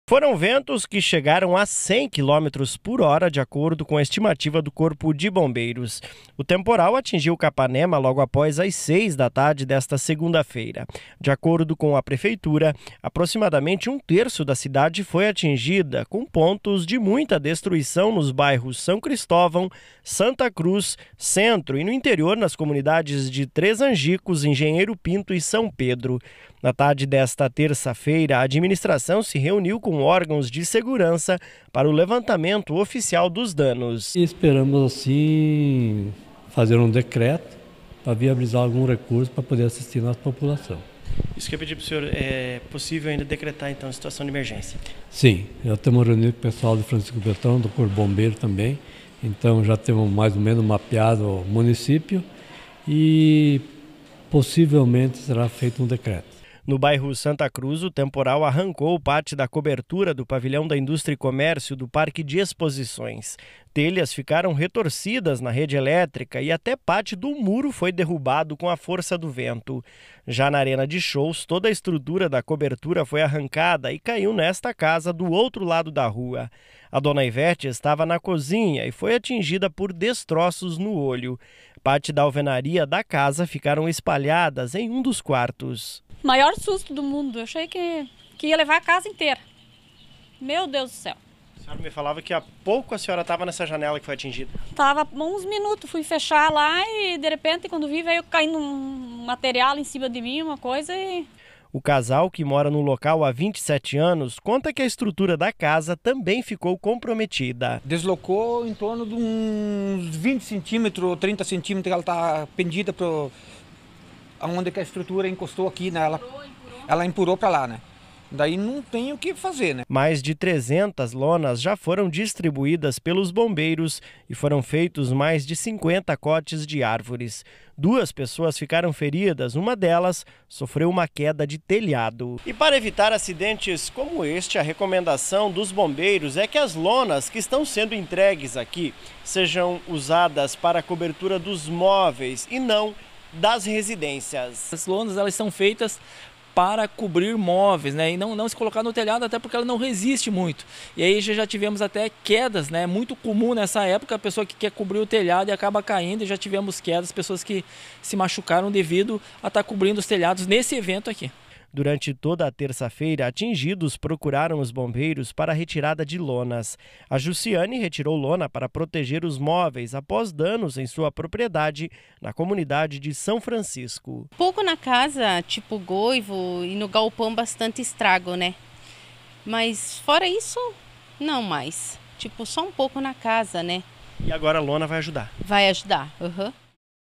Temporal destrói casas e derruba árvores em Capanema
Clima: Duas pessoas ficaram feridas e os bombeiros distribuíram mais 300 lonas.